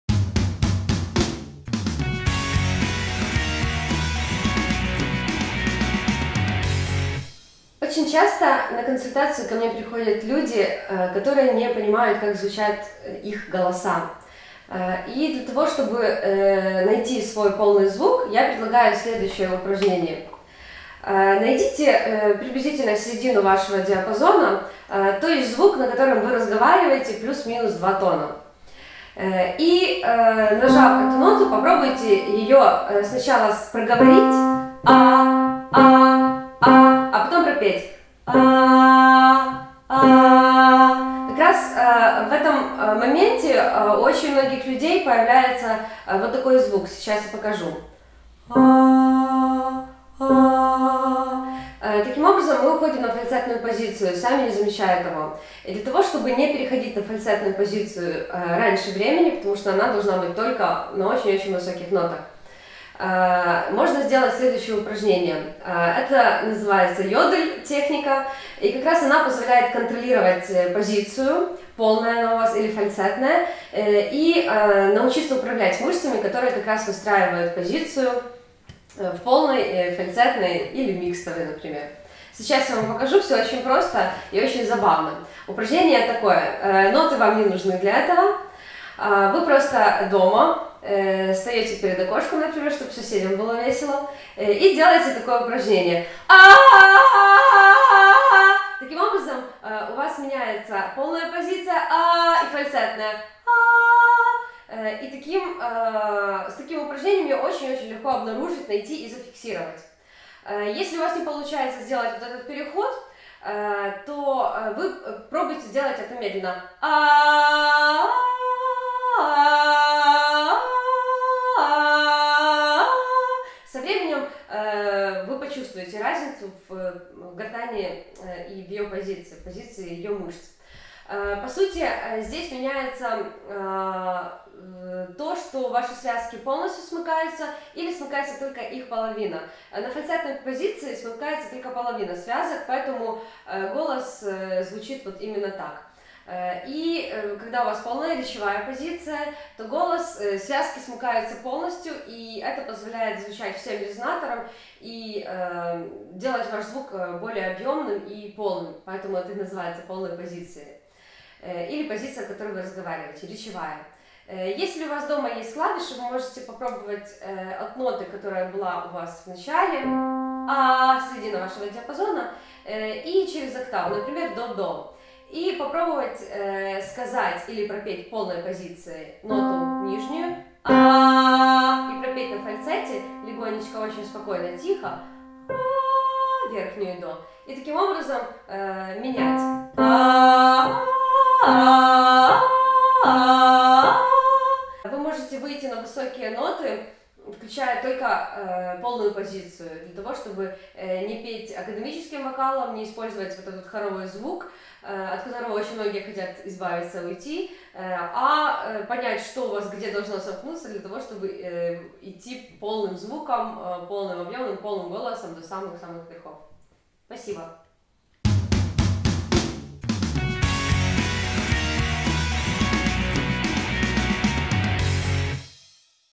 Речевая позиция. Грудной голос. Йодль. Уроки вокала Ирины Лещик-Третьяковой
Уроки вокала от Ирины Лещик-Третьяковой.
Речевая позиция. Грудной голоса. Йодль.

В своем видео-уроке вокала Ирина рассказывает как петь в речевой позиции (петь как говоришь). Упражнение, которое показывает Ирина в своем уроке вокала используется техника йодль, которая помогает научиться переходить с полной позиции на фальцет и обратно и научиться петь в речевой позиции.

Смотрите урок вокала от Ирины Лещик-Третьяковой "Речевая позиция. Грудной голос. Йодль".